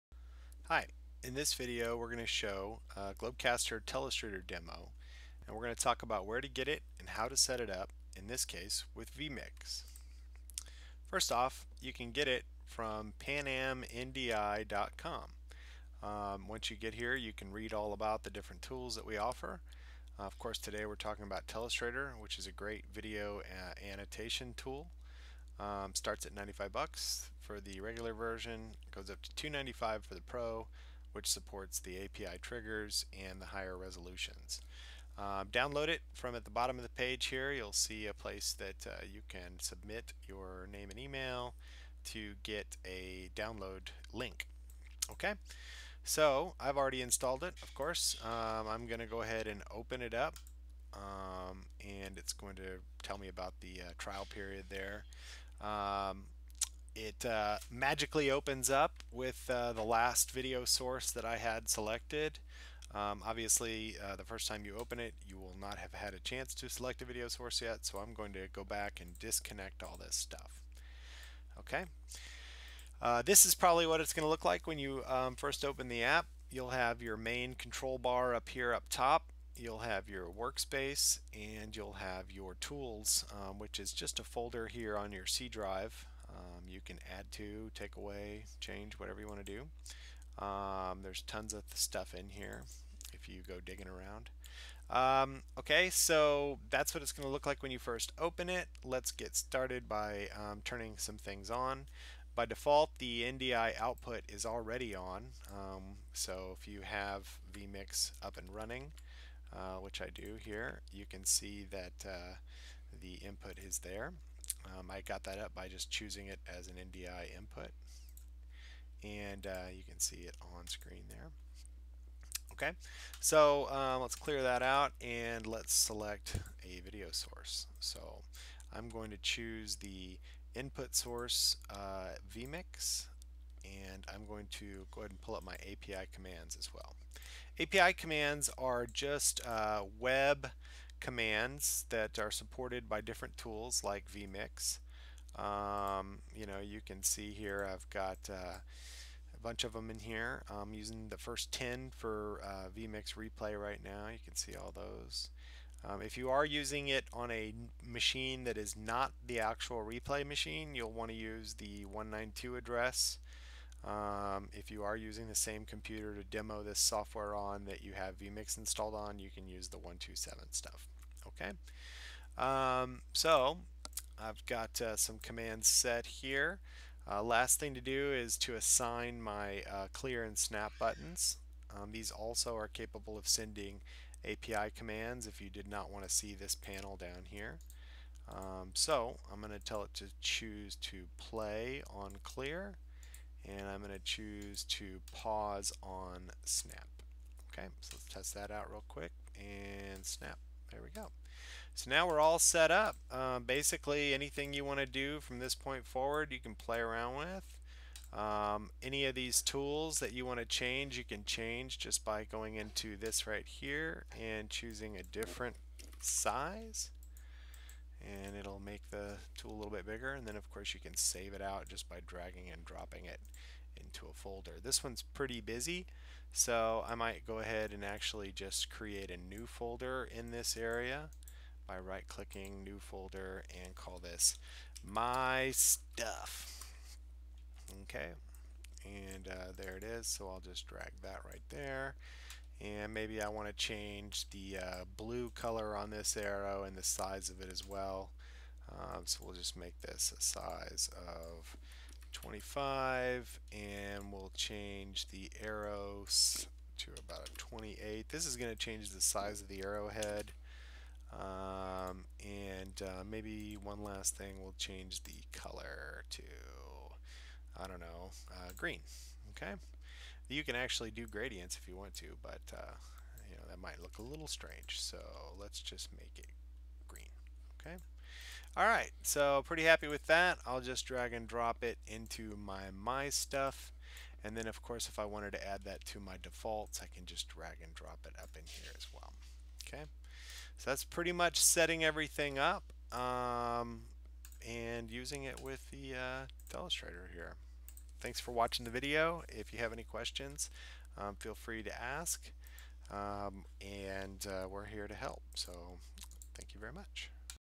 5 Minute Telestrator Demo
In this demo we show where to get GlobeCaster Telestrator and how to set it up to work with vMix over NDI and with API commands.